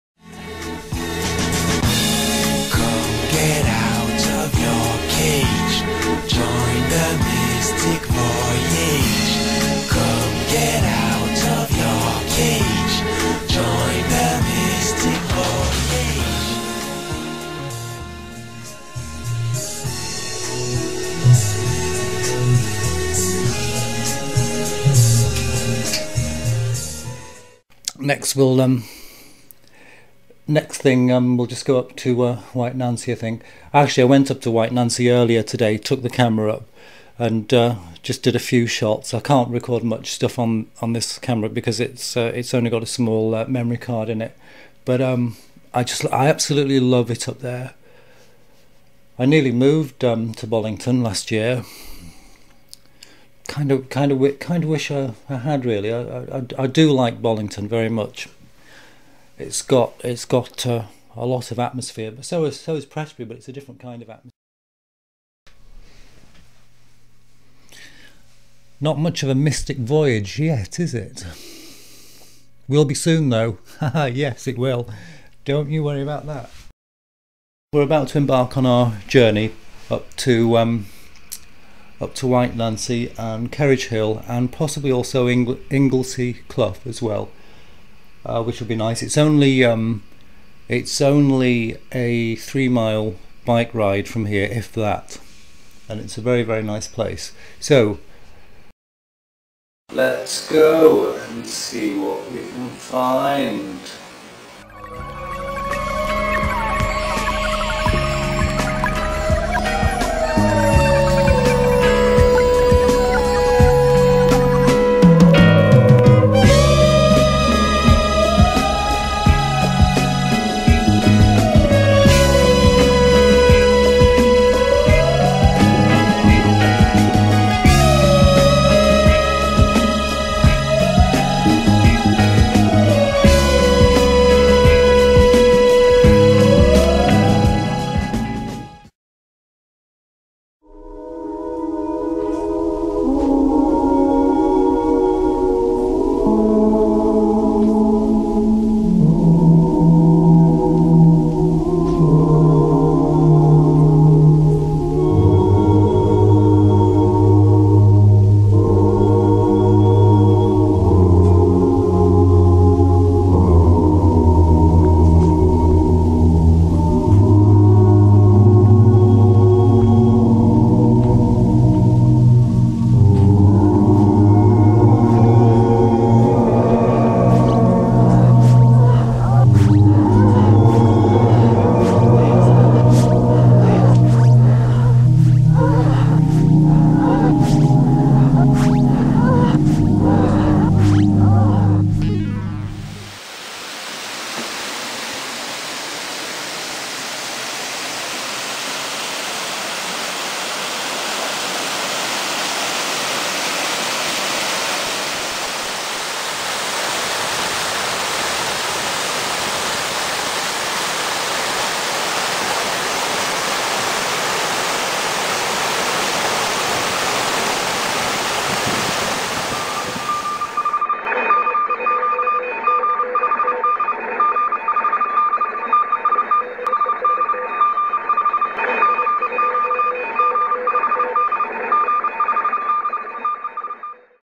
White Nancy Bollington
Voyage to White Nancy, Bollington Cheshire. A LoFi experimental movie paying homage to White Nancy in Bollington, one of my favorite local places. This was shot on a Sony W50 with separate sound recorded directly to the computer then re-synced with video in Movie Maker.